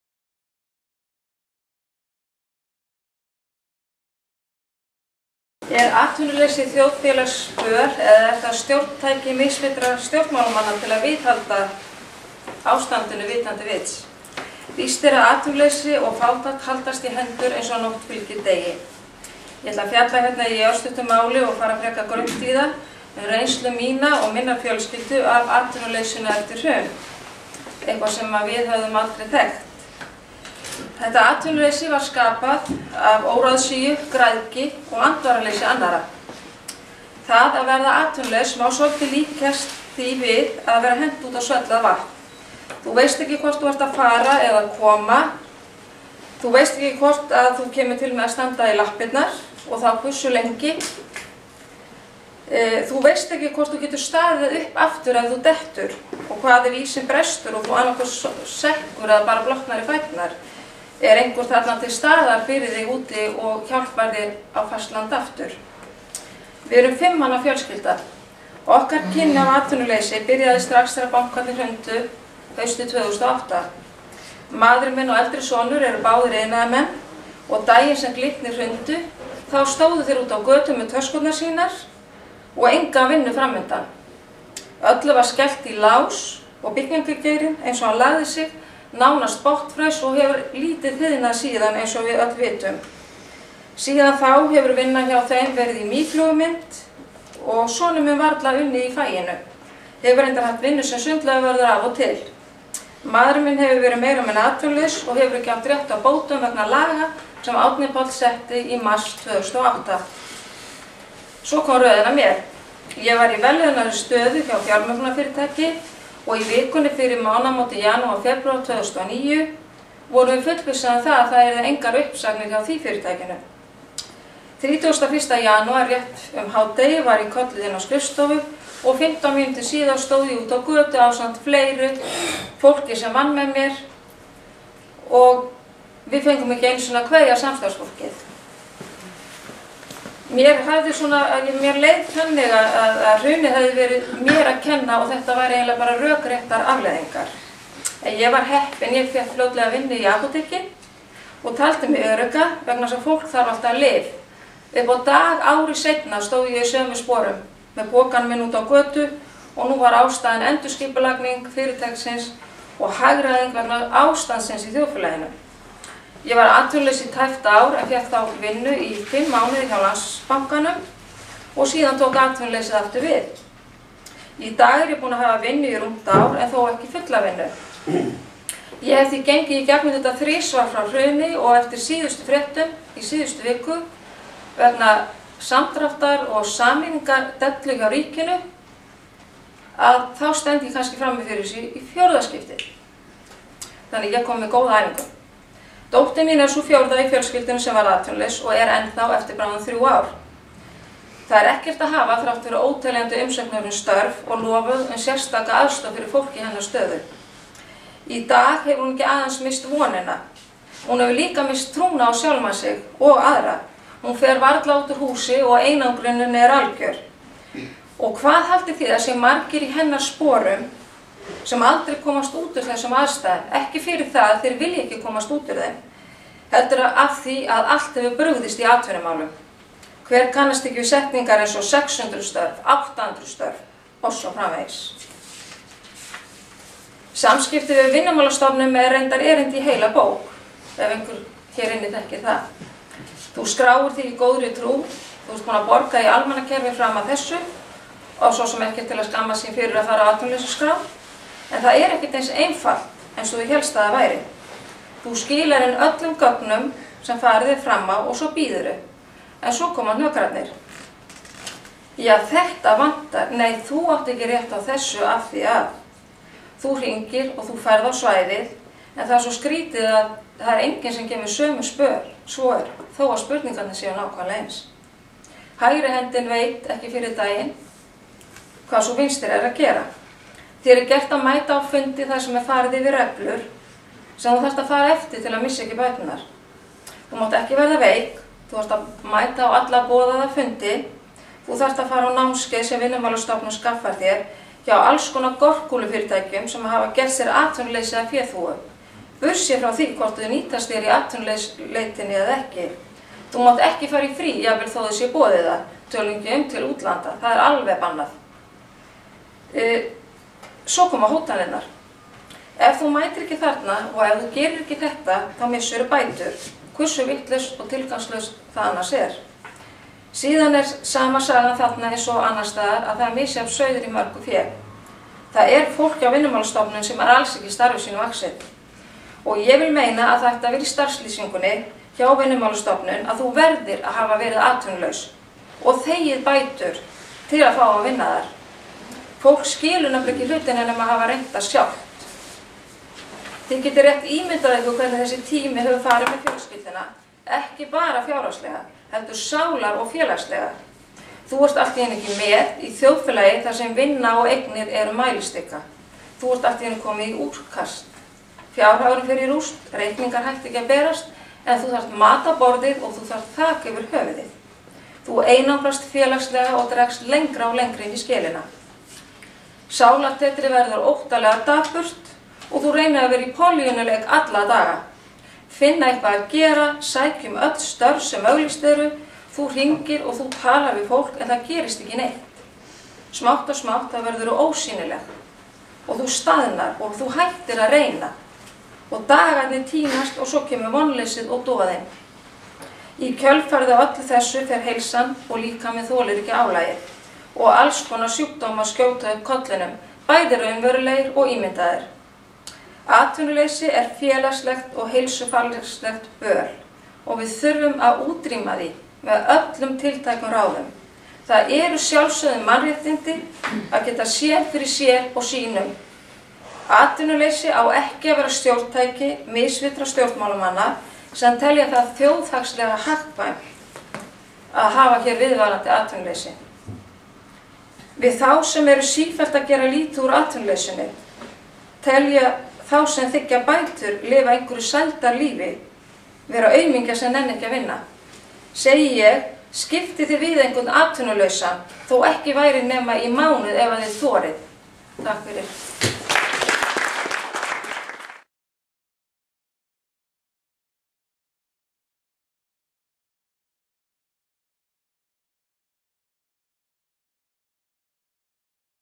Fátækt er fjötur sem þarf að leysa
Pálmey H. Gísladóttir segir frá kjörum fjölskyldu sinnar sem hefur öll upplifað atvinnuleysi í lengri og skemmri tíma frá hruni.